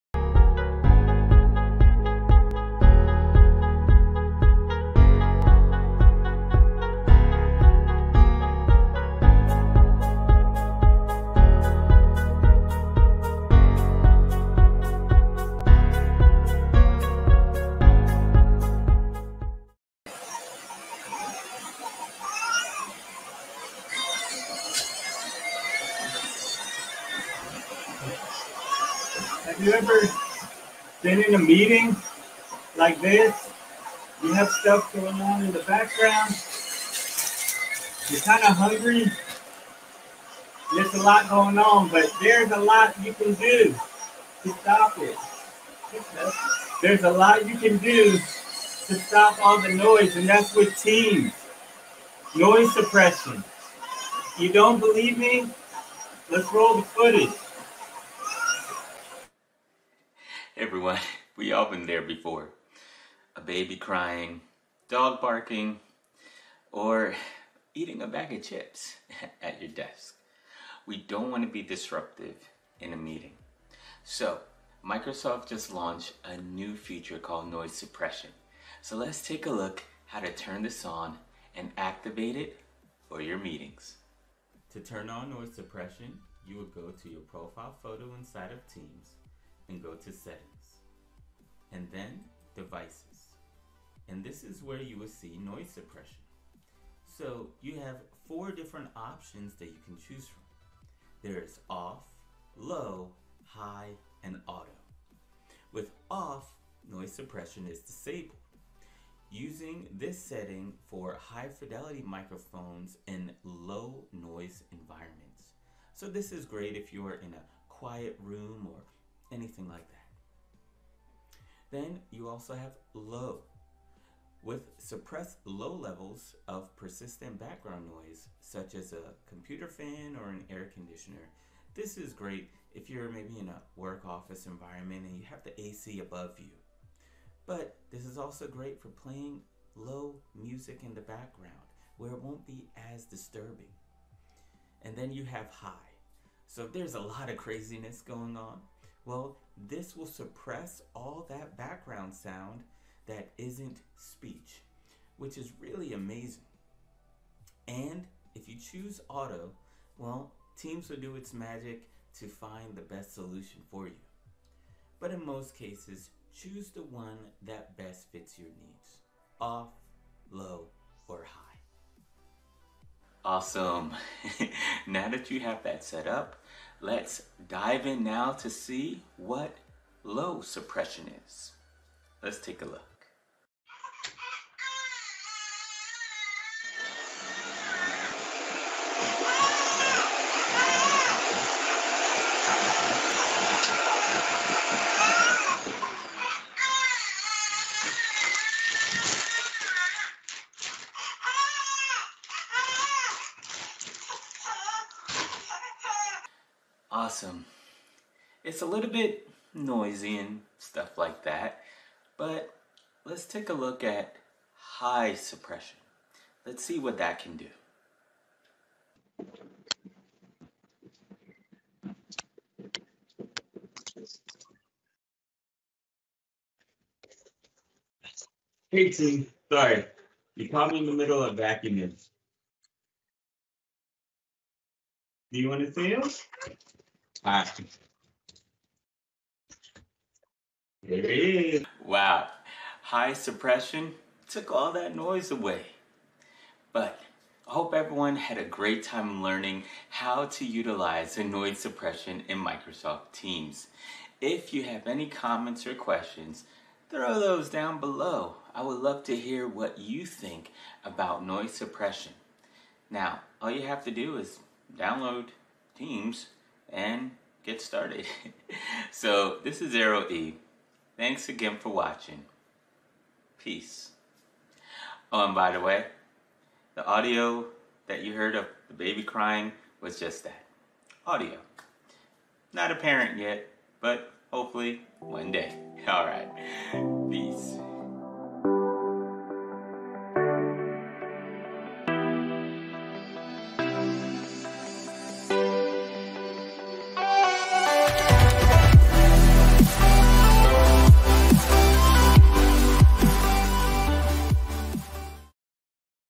Noise Suppression In Microsoft Teams
A step-by-step video on how to reduce background noise in Microsoft Teams meetings with Noise Suppression. Noise Suppression in Microsoft Teams meetings has now rolled out and can help cut out or reduce background sounds coming through on your end of the meeting. Whether it is a vacuum cleaner, bag of chips or a barking dog, Microsoft Teams Noise Suppression is great for remote working and school. Noise Suppression in Teams is currently only available in windows Desktop.

In the near future, the Noise Suppression setting will also be available in the "..." | Device Settings area of the Meeting window as well, so you can change the Noise Suppression setting and remove background noise with Microsoft Teams in real-time.